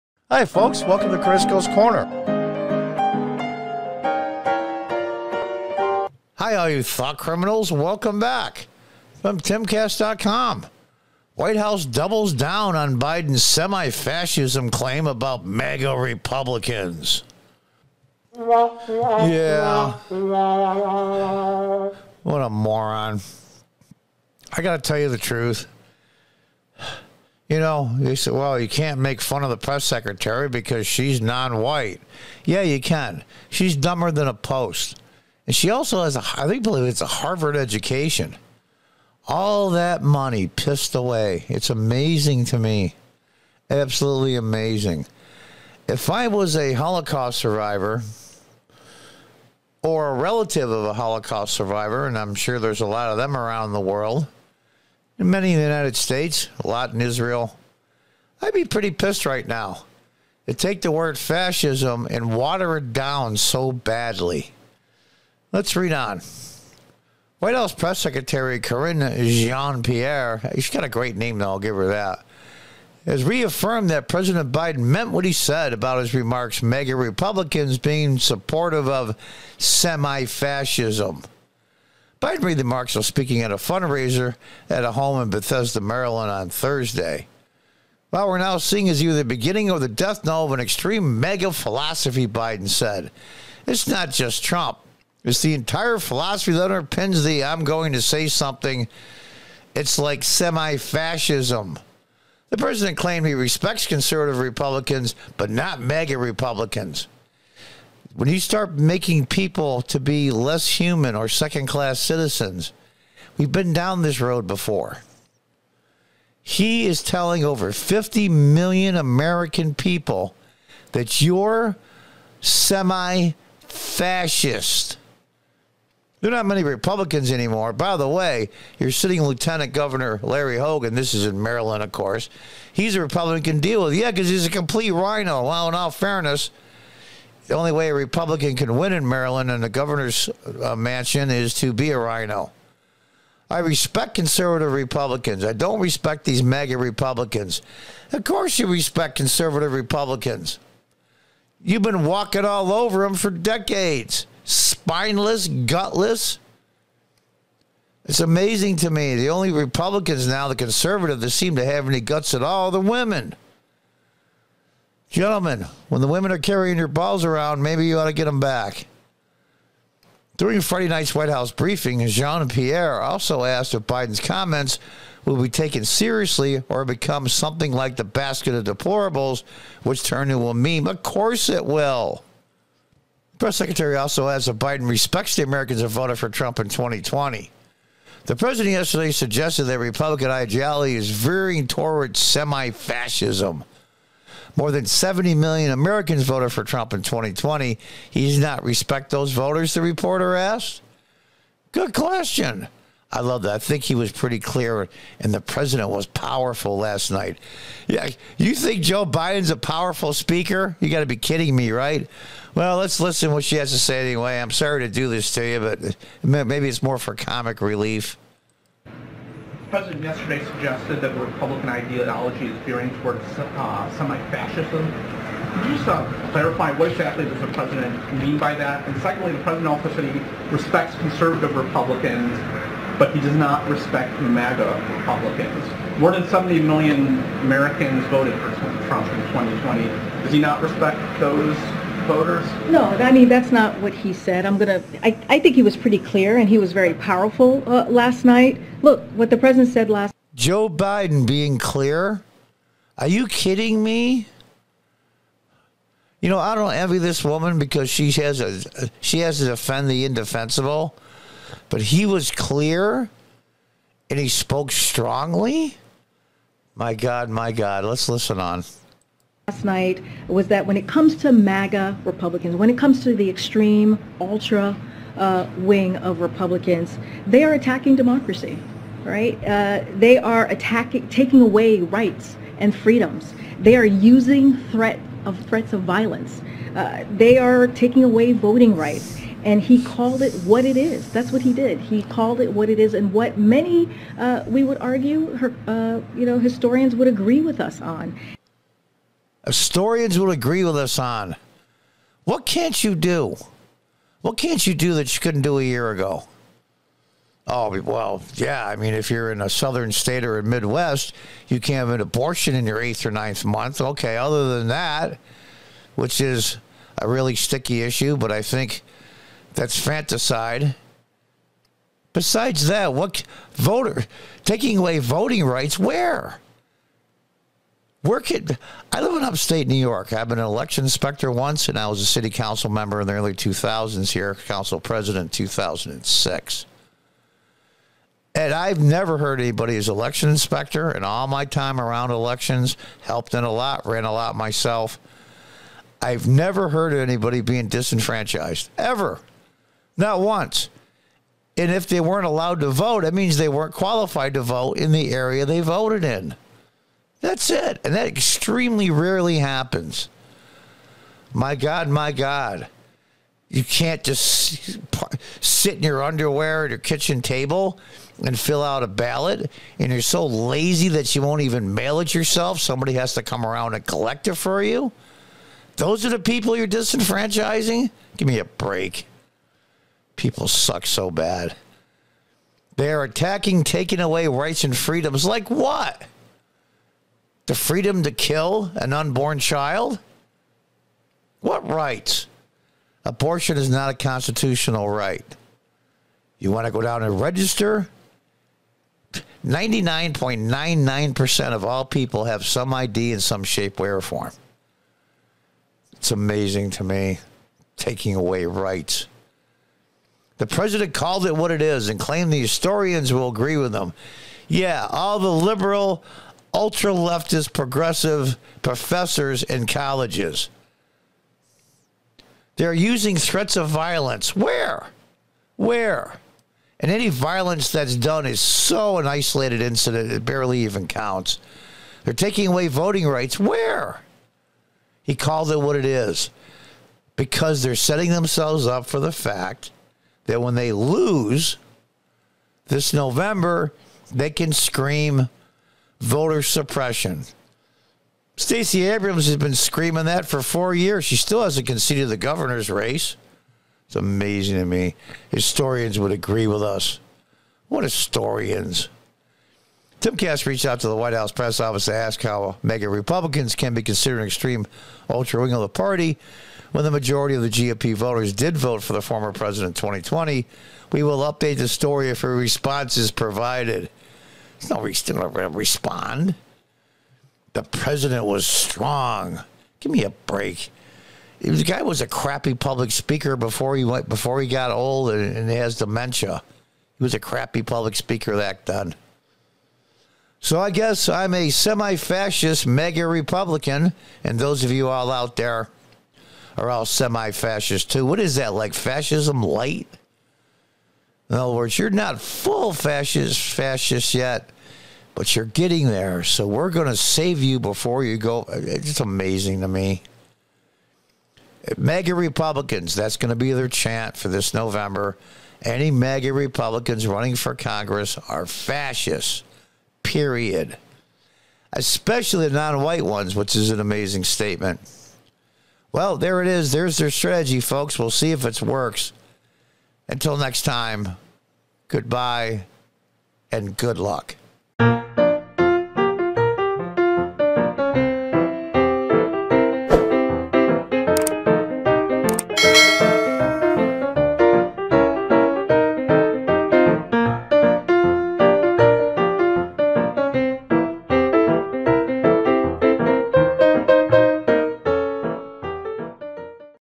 Biden: MAGA Republicans Semi Fascists
Joe Biden called "MAGA" Republicans semi-fascists. So now 70+ million people are now semi fascists. Interesting political strategy. These people suck so bad.